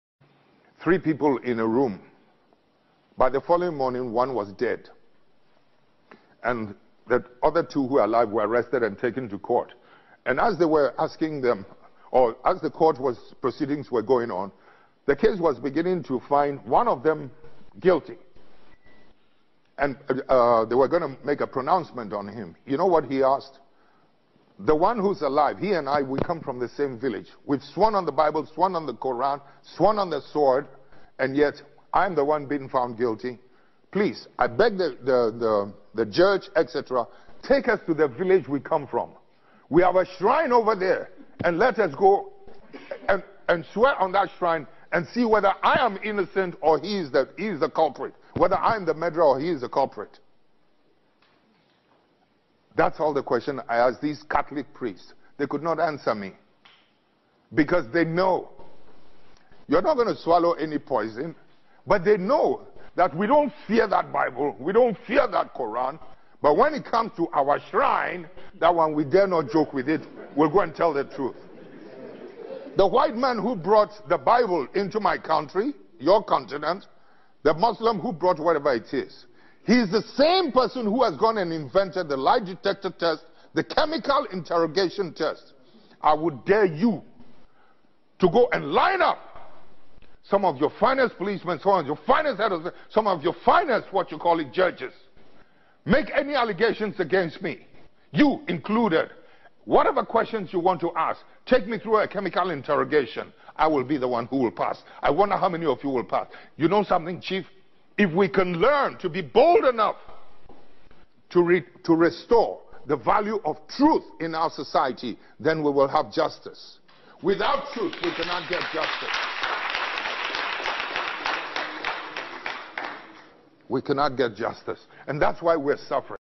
False, pagan, Europeanised, romanised religion ruined Africa sir JJ RAWLINGS
Sir JERRY John Rawlings, great pan Africanist ✊🏾. Rest in power sir! pray for us 🤲🏾👌🏾.